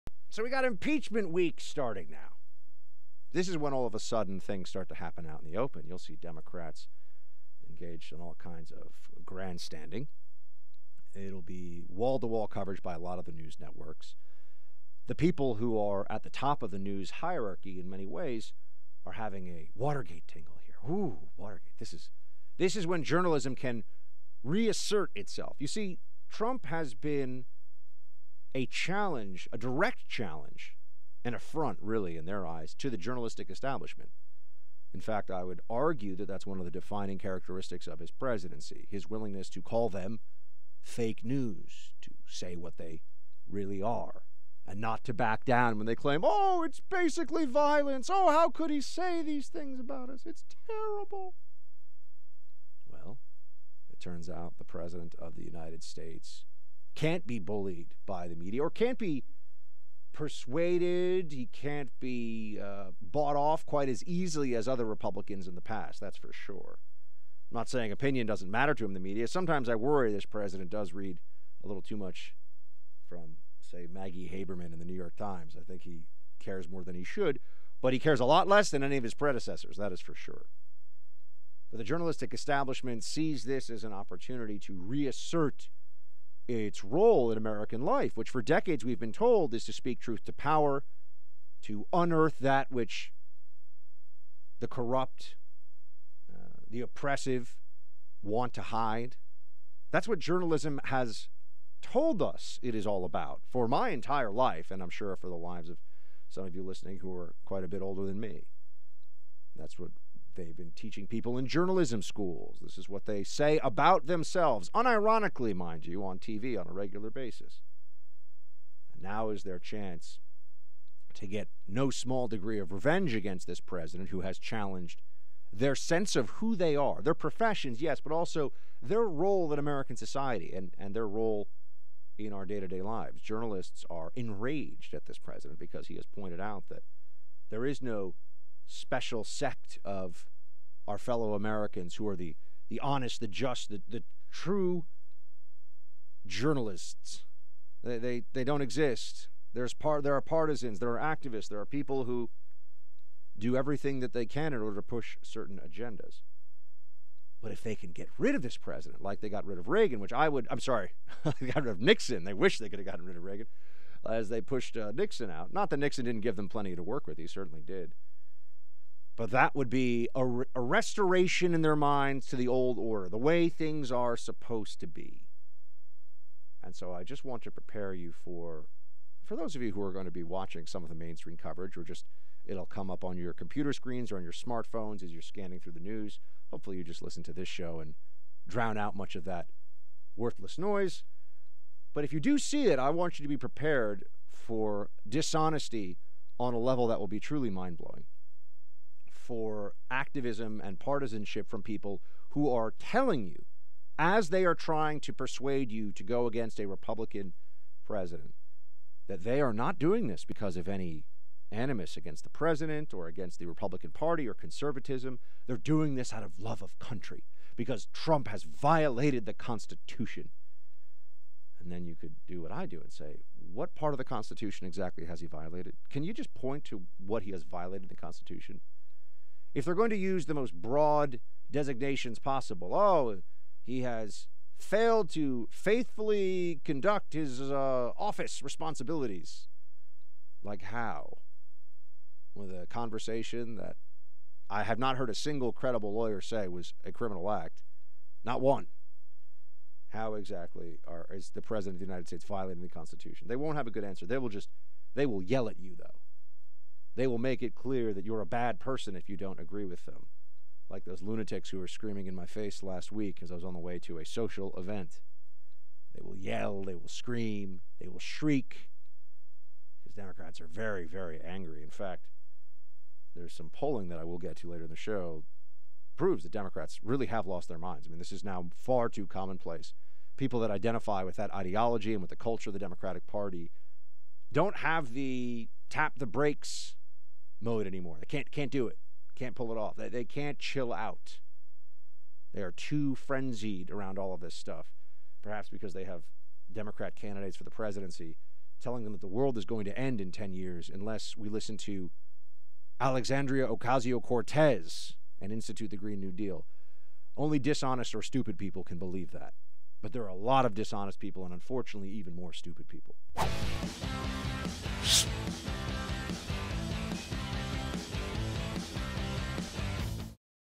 What To Expect for “Impeachment Week”
Get ready for impeachment insanity in the media this week.